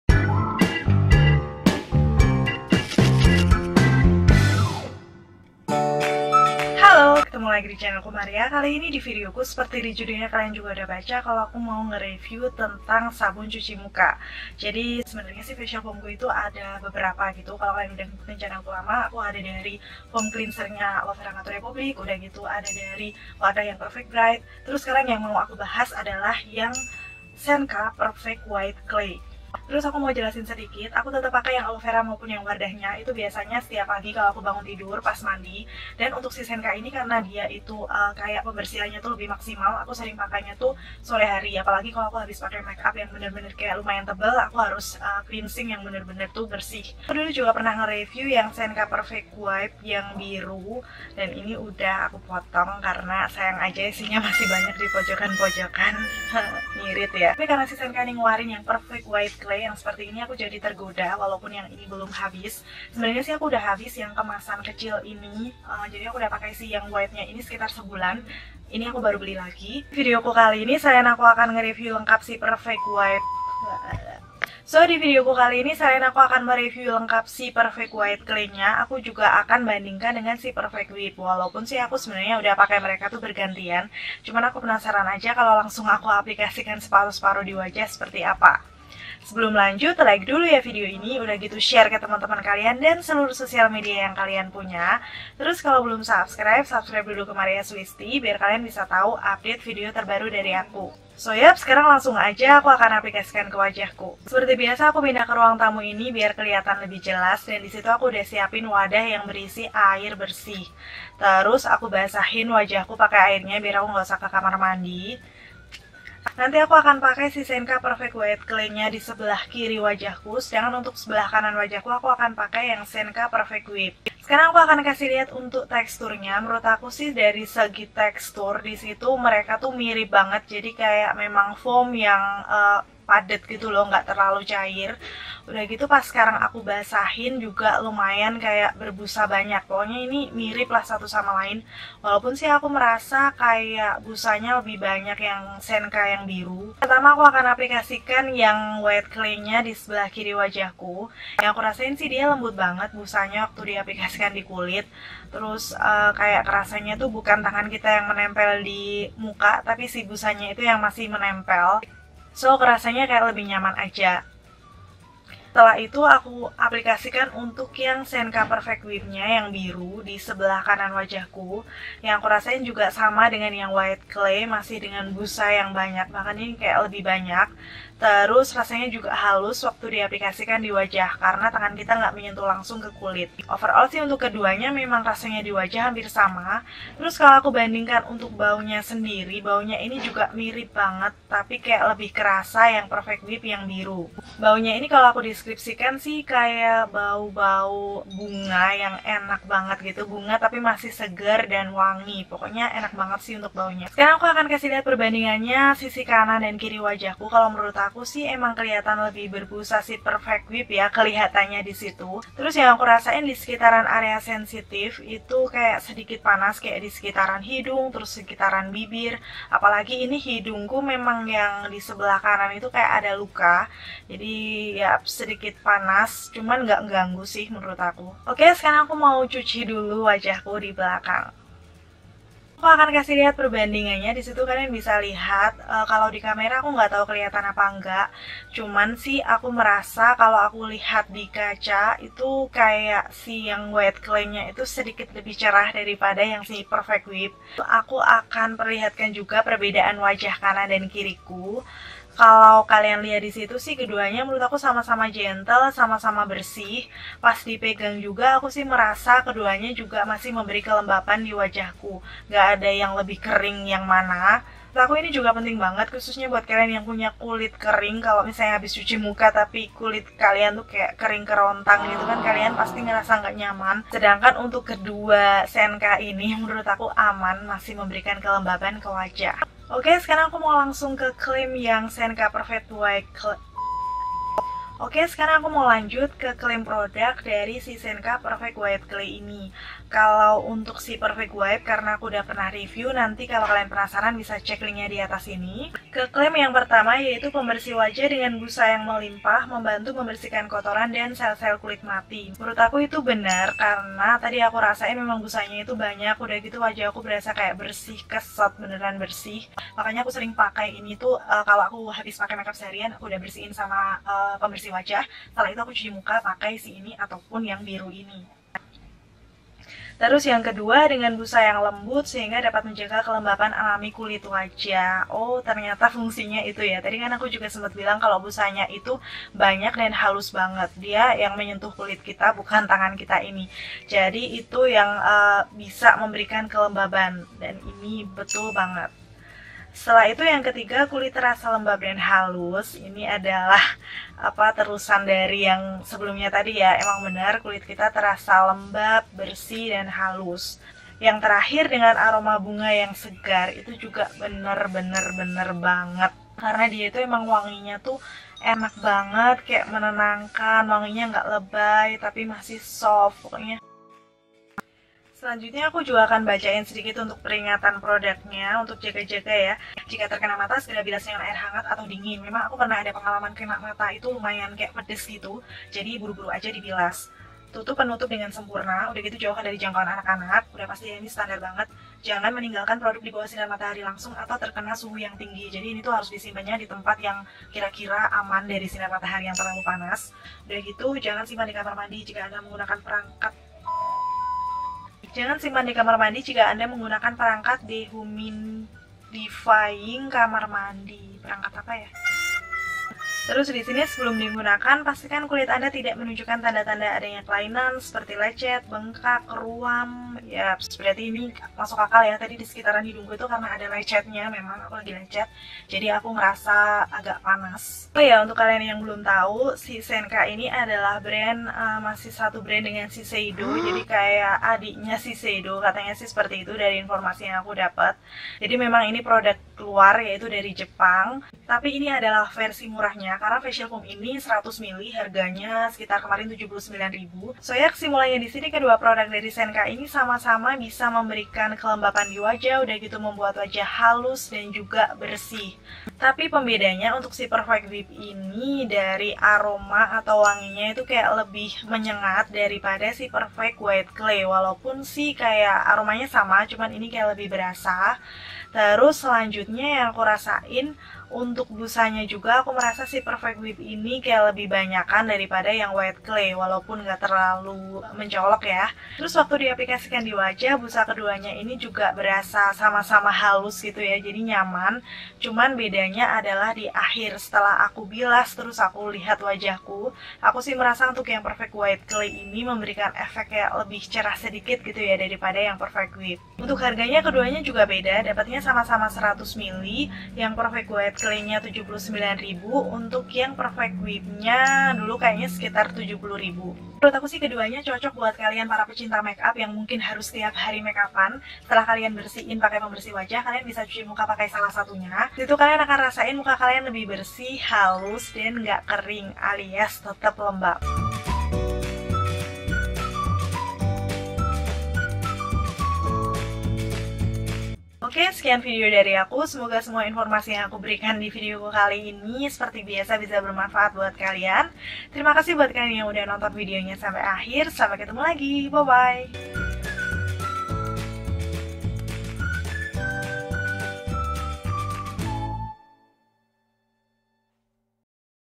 SENKA PERFECT WHITE CLAY REVIEW | BEDANYA SAMA SENKA PERFECT WHIP? |Maria Soelisty
Hai dears,
di sini aku mau share pengalamanku pakai 2 produk sabun cuci muka dari Senka yg Perfect White Clay & Perfect Whip..
kalau dirasain sih hampir ga ada bedanya, tapi ternyata beda lho..😅
Yang pasti buat kalian yang suka pakai makeup tiap hari, produk ini sangat recomended karena bisa ngebersihin yan beneran bersih, tanpa buat kering (tetap lembab)😍
Semoga informasinya bermanfaat 😉

Q&A :
- Ka, tipe kulitnya apa?
👧 tipe kulitku kombinasi, area TZone berminyak, area lain normal ke kering
- Ka, skintonenya apa?
👧 skintone medium aga kuning langsat
- Ka, pakai kamera apa?
👧 Canon G7X